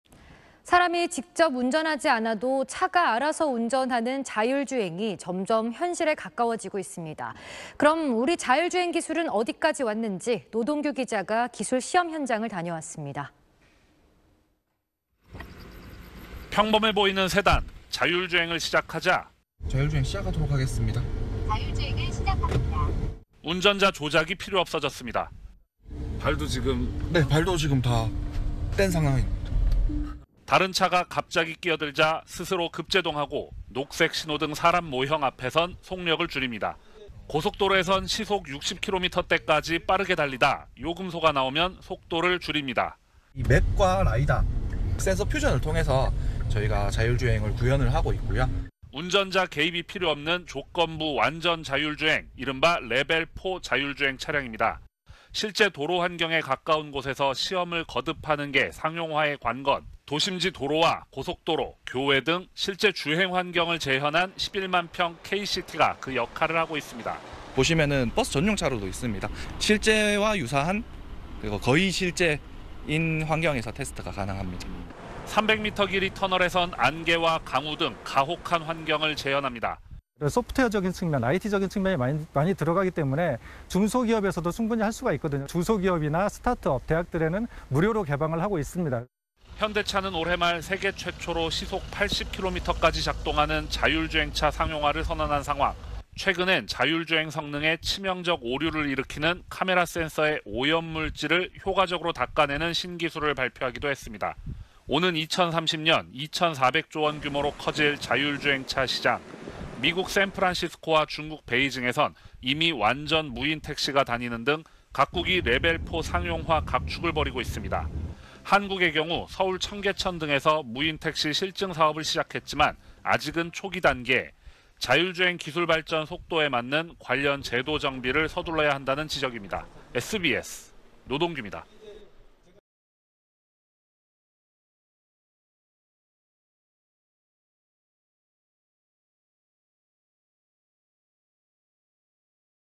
끼어들고 비 뿌리고…현실 가까워진 자율주행차, 시험해 보니 / SBS
〈앵커〉

사람이 직접 운전하지 않아도 차가 알아서 운전하는 자율주행이, 점점 현실에 가까워지고 있습니다.

우리 자율주행 기술은, 어디까지 왔는지 노동규 기자가 기술 시험 현장을 다녀왔습니다.

〈기자〉

평범해 보이는 세단, 자율주행을 시작하자,

[자율주행 시작하도록 하겠습니다.]

운전자 조작이 필요 없어졌습니다.

[(발도 지금?) 네, 발도 지금 다 뗀 상황입니다.]

다른 차가 갑자기 끼어들자 스스로 급제동하고, 녹색 신호등 사람 모형 앞에서는 속력을 줄입니다.

고속도로에서는 시속 60km대까지 빠르게 달리다 요금소가 나오면 속도를 줄입니다.

[지도와 라이다, (카메라, GPS 등) 센서 퓨전(데이터 통합)을 통해서 자율주행을 구현하고 있고요.]

운전자 개입이 필요 없는 조건부 완전자율주행, 이른바 '레벨4' 자율주행차량입니다.

실제 도로 환경에 가까운 곳에서 시험을 거듭하는 것이 상용화의 관건.

도심지 도로와 고속도로, 교외 등 실제 주행 환경을 재현한 11만 평의 'K시티'가 그 역할을 하고 있습니다.

[김기렴/오토노머스에이투지 매니저 : 보시면 버스전용차로도 있습니다. 실제와 유사한, 거의 실제인 환경에서 시험이 가능합니다.]

300m 길이 터널에서는 안개와 강우 등 가혹한 환경을 재현합니다.

[최인성/자동차안전연구원 K시티 연구처장 : 소프트웨어, IT 기술이 많이 들어가기 때문에 중소기업에서도 충분히 할 수 있거든요. 중소기업이나 스타트업, 대학들에는 무료로 개방하고 있습니다.]

현대차는 올해 말 세계 최초로 시속 80km까지 작동하는 자율주행차 상용화를 선언한 상황.

최근에는 자율주행 성능에 치명적 오류를 일으키는 카메라 센서의 오염 물질을 효과적으로 닦아내는 신기술을 발표하기도 했습니다.

오는 2030년 2천400조 원 규모로 커질 자율주행차 시장.

미국 샌프란시스코와 중국 베이징에서는 이미 완전 무인 택시가 다니는 등 각국이 레벨4 상용화 각축을 벌이고 있습니다.

한국의 경우 서울 청계천 등에서 무인 택시 실증사업을 시작했지만, 아직은 초기 단계.

자율주행 기술 발전 속도에 맞는 관련 제도 정비를 서둘러야 한다는 지적입니다.

(영상취재 : 최대웅, 영상편집 : 신세은)